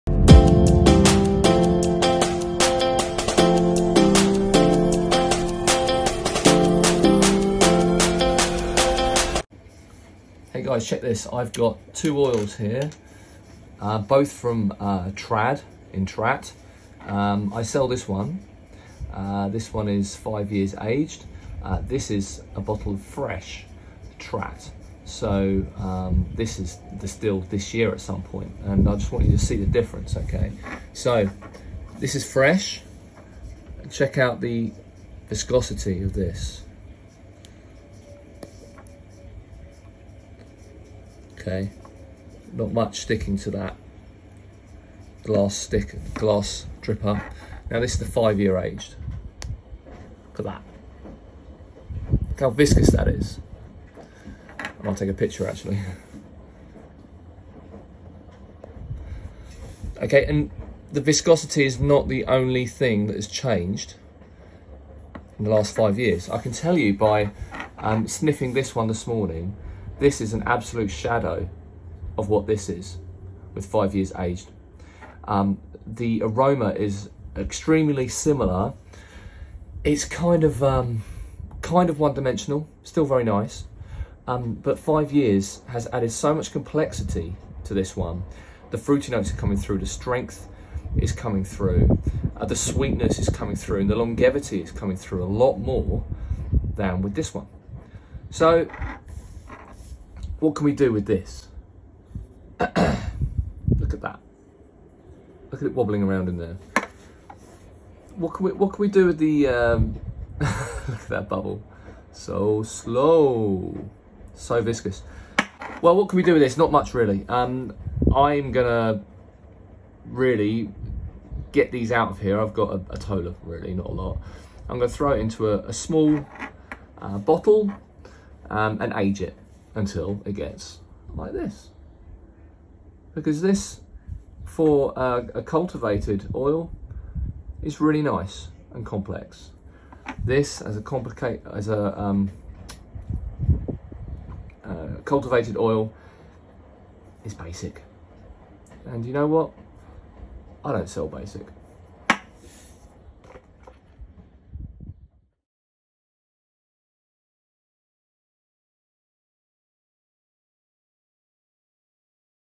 Aged Oud Vs Fresh Oud Oil
What is the difference between fresh Oud and aged Oud? Here we compare 2 Trat ouds from the same farm. One is fresh, the other has been aged for 5 years. The trick in ageing Oud is to keep it in a bottle  that is at 70% capacity. Every month leave the cap open for the Oud to breath. This method gives the Oud enough oxygen to age properly. Oud is an oil distilled from the resin from Aquilaria species tree found in Asia. The resin is formed either in the wild by natural cuts in the wood made by bugs or managed by man in farms using various techniques. The resin is a result of the tree protecting itself from fungus attack. Oud is very expensive to produce and commands a high price!